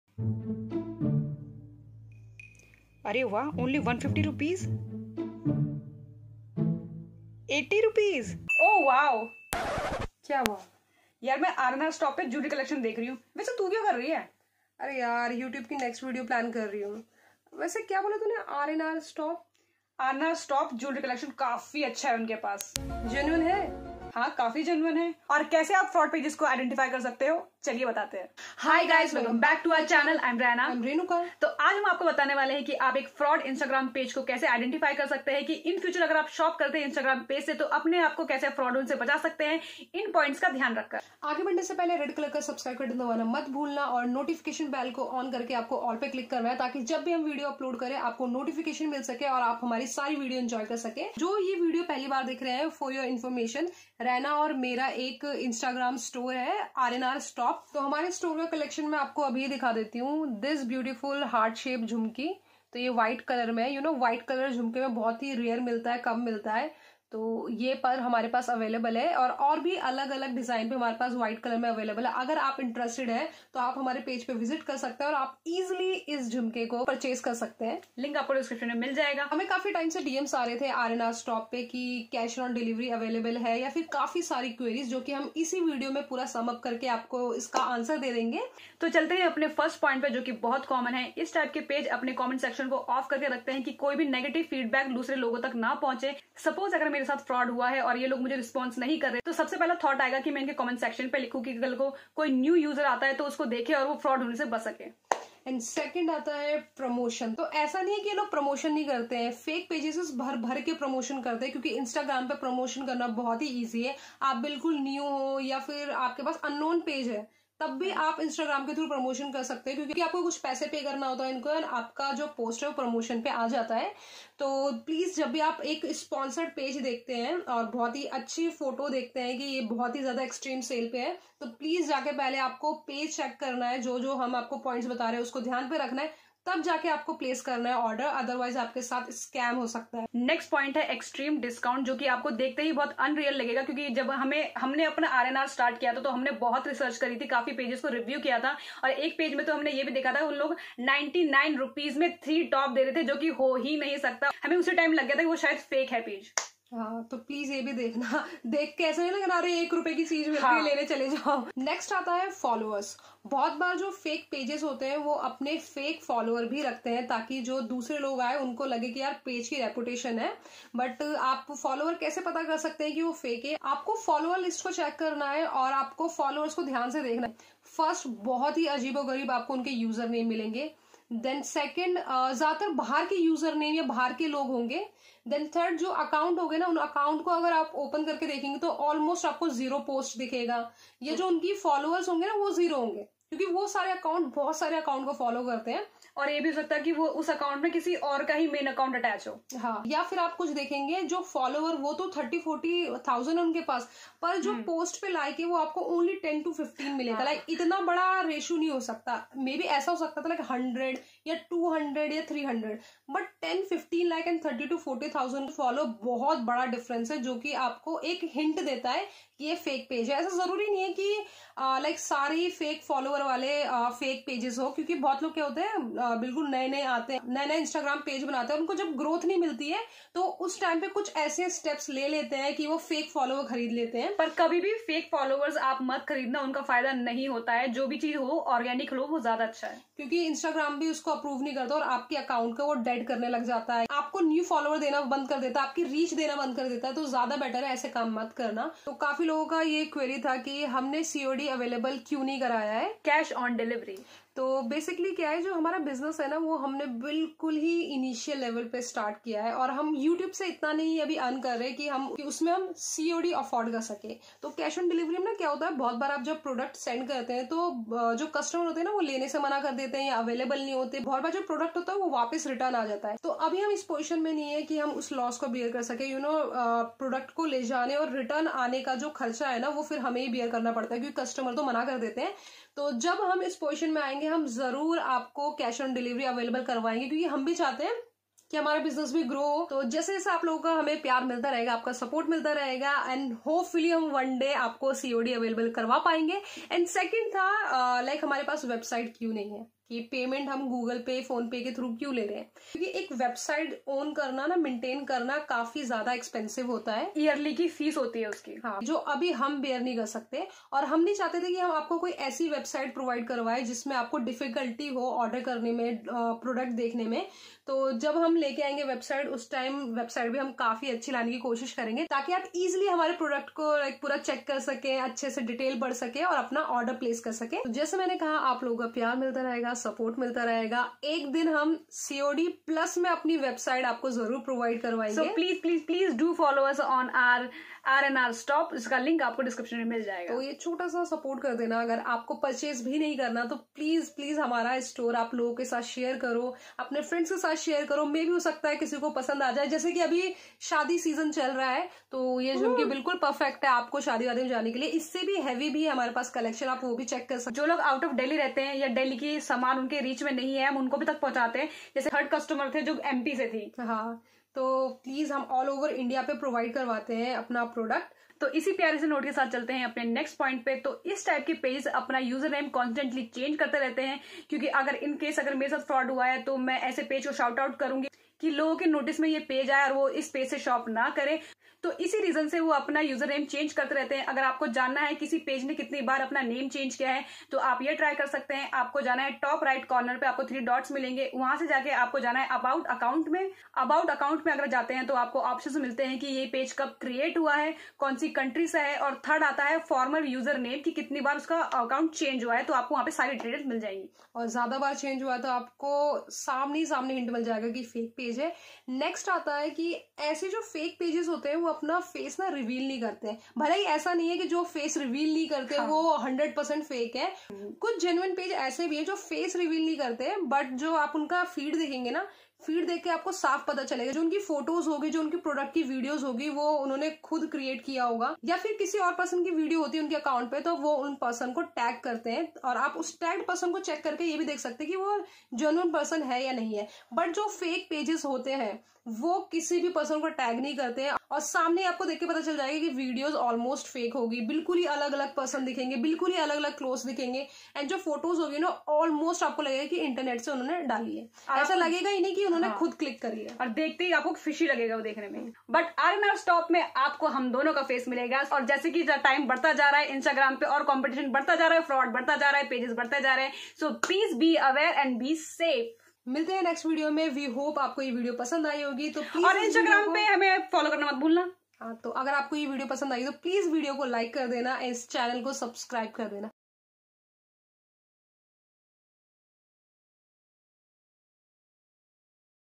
Fake Instagram Stores | Big Money SCAM , Fraud vs Real online shops || Raina and Renuka
We have explained some points
How to identify fake online instagram stores?
Aware from fraud shops
Videos-
Phone we use to shoot